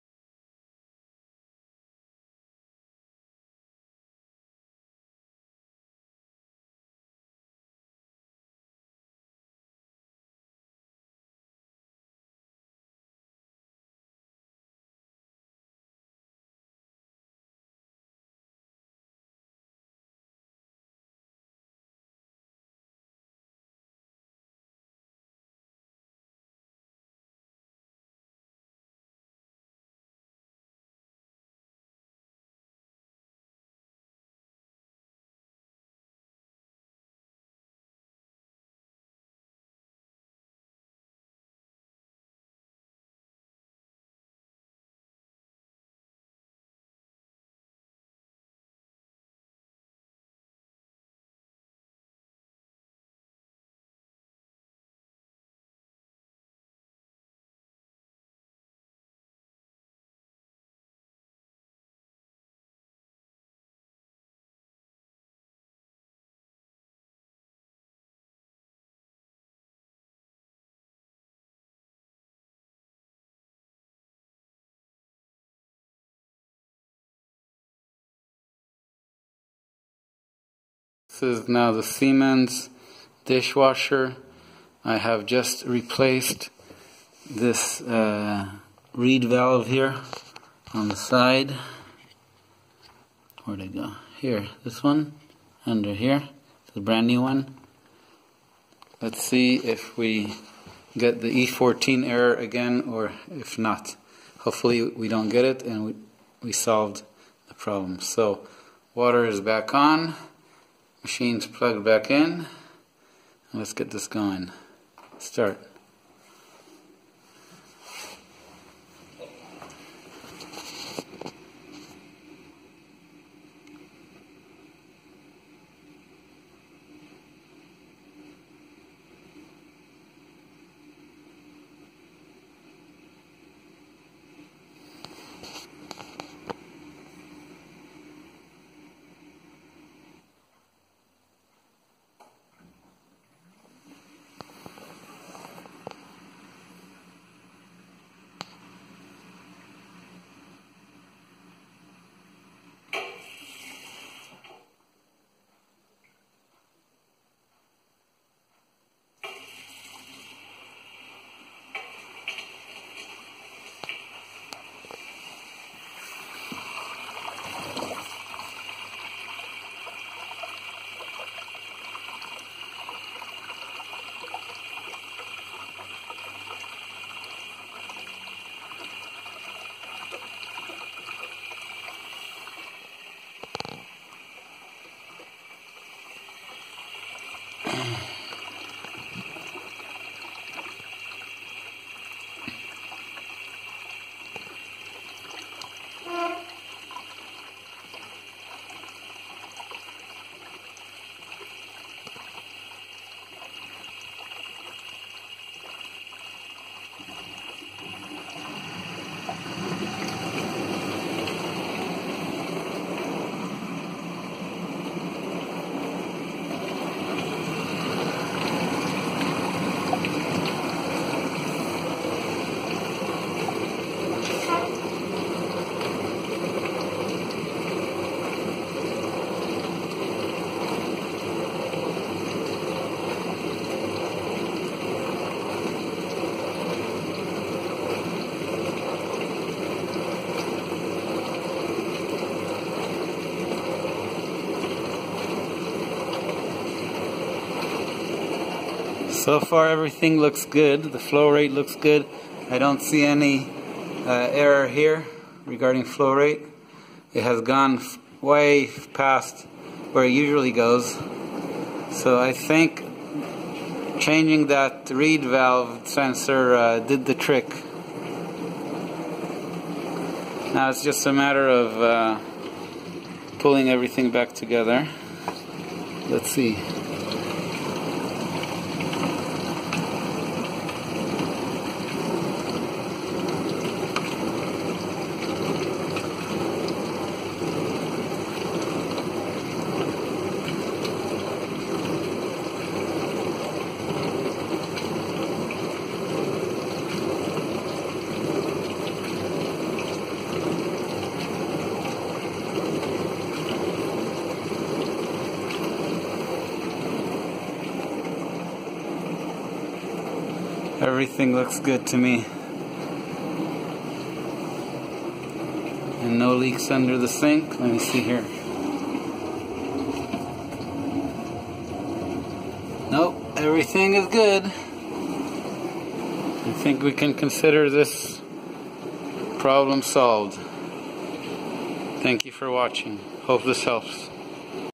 How to fix a dishwasher showing the E14 error code
The E14 error code in Siemens and Bosch dishwashers is most often caused by a faulty sensor whose function is to detect the flow rate of water coming into the machine. This sensor (Part no. 00611317) can be easily replaced. In this video, I show you how.